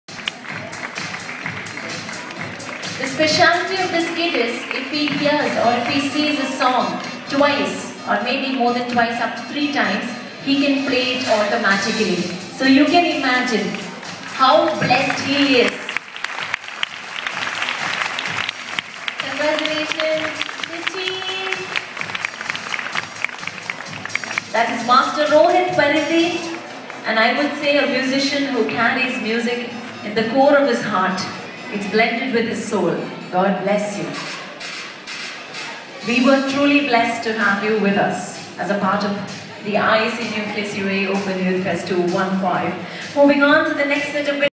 Rohith's performance with keyboard
Rohith won the jury award for his wonderful performance in keyboard -UAE youth festival held at India Social And Cultural center  Abudhabi